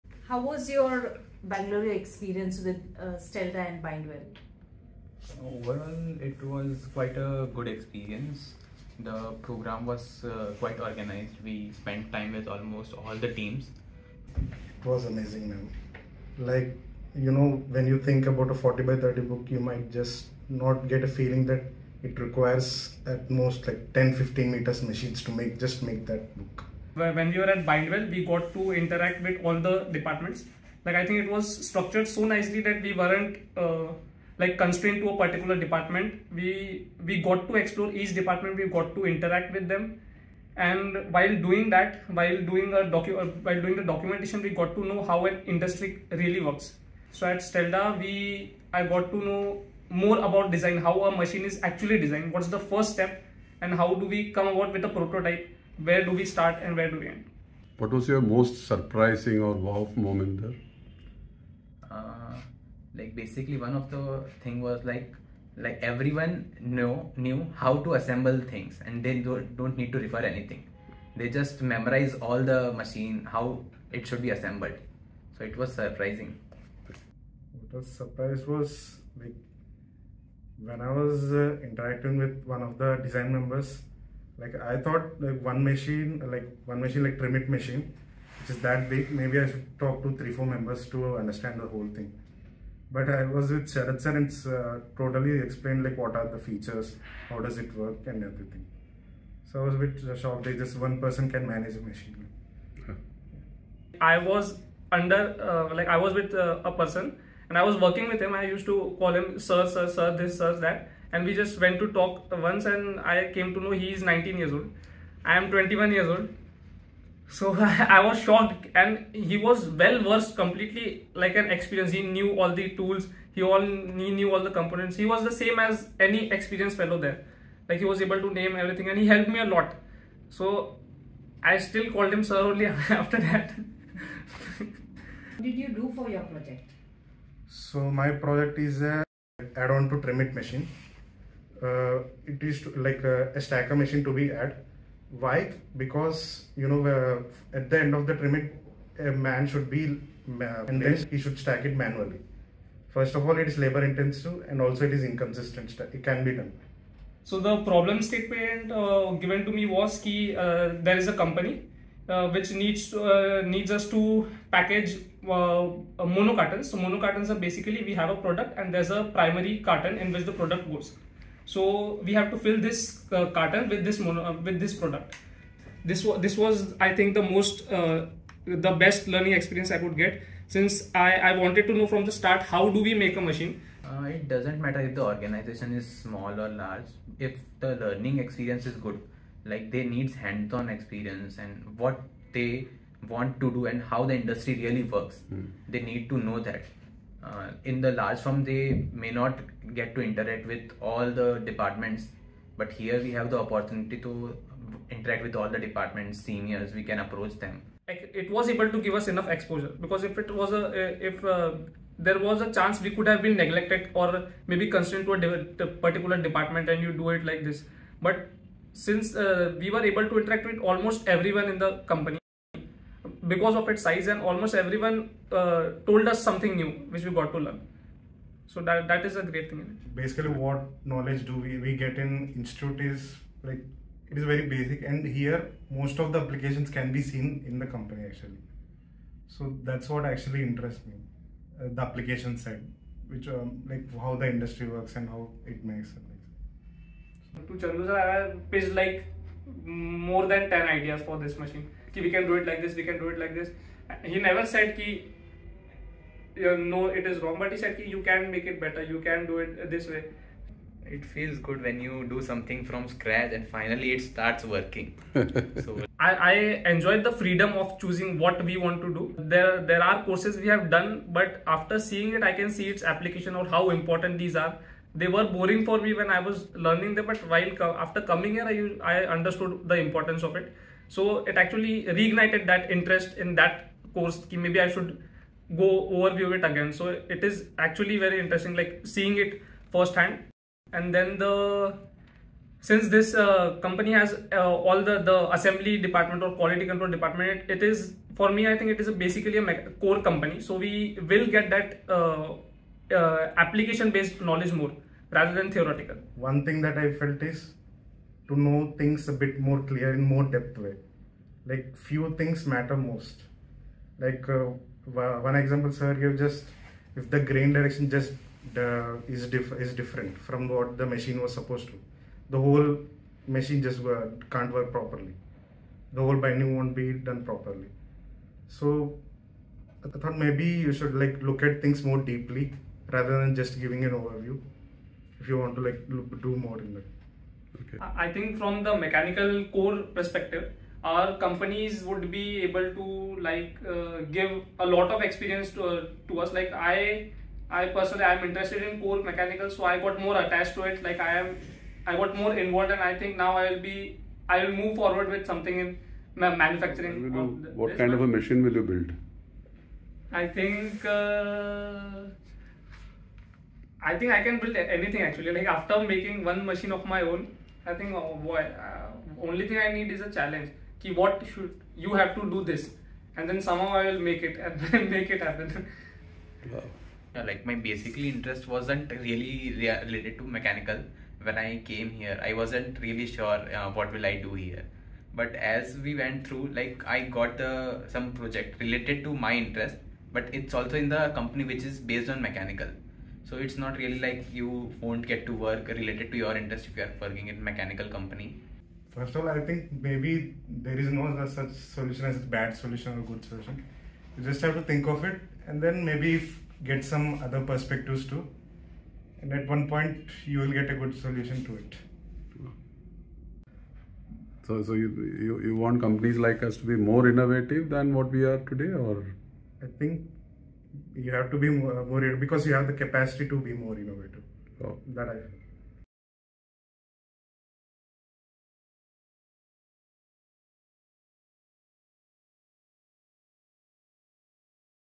Interview with IIT Bombay students who interned with Bindwel and Stelda
3 students from Indian Institute of Technology, Bombay - which is ranked 1st in India among Engineering Colleges, chose Bindwel and Stelda for their Internship. They were introduced to a unique Internship program developed by these companies, belonging to the Impel group. The 6 weeks program introduced them to getting expertise in processes and provided industry exposure. In the end each of them were given a project - based on a problem statement for which they attempted to find solutions.
They share their experiences, learnings, interesting moments spent with one of the largest manufacturers of bookbinding machines in the world "Bindwel" and a Start up in end of line packaging "Stelda"